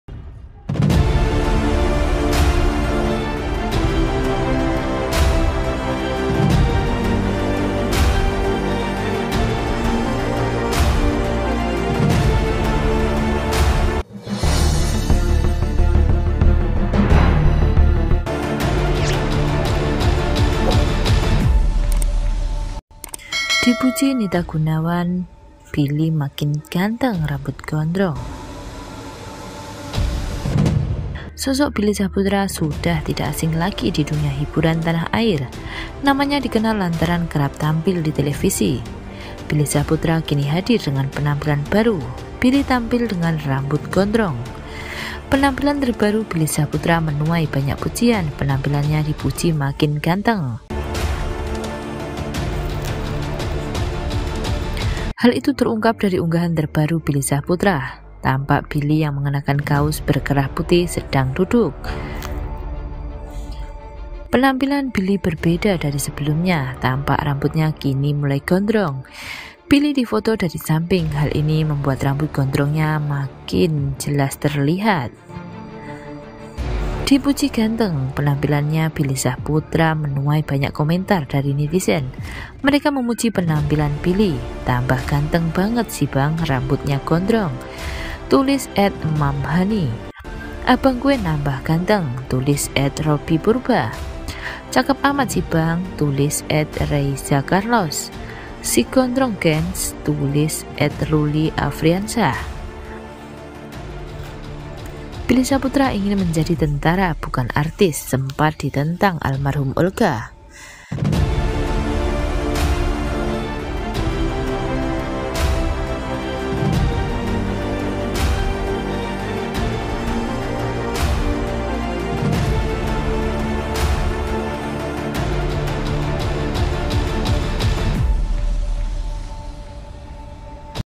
Penampilan Terbaru Billy Syahputra dengan Rambut Gondrong, Dipuji Makin Ganteng
Potret Terbaru Billy Syahputra dengan Rambut Gondrong, Dipuji Makin Ganteng